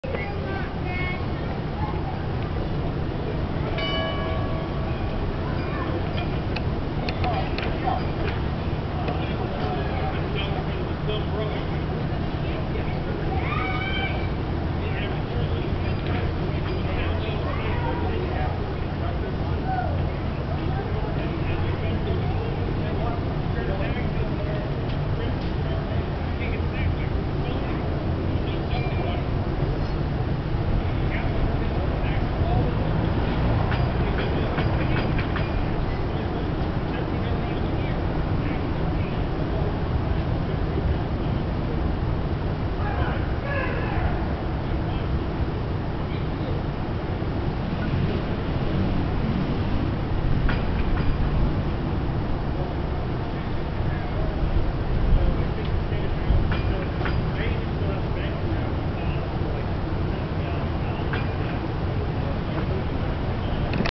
BSM Car #4533, All Aboard! a 1904 Brill Streetcar
Here is video from the Baltimore Streetcar Museum, as the weekend passengers are welcomed on board Streetcar # 4533. Streetcar # 4533 is a United Railway and Electric Company model, built by J.G. Brill in 1904. Many families line up to take an educational spin around the BSM tracks. Open Sundays all year round, the BSM is the place to be if you want to take a ride down Nostalgia Road. The BSM is a blast for kids also, as you will see, because they are under the tutelage of Baltimore's finest Conductors, skilled in the transportation of passengers through Streetcar History.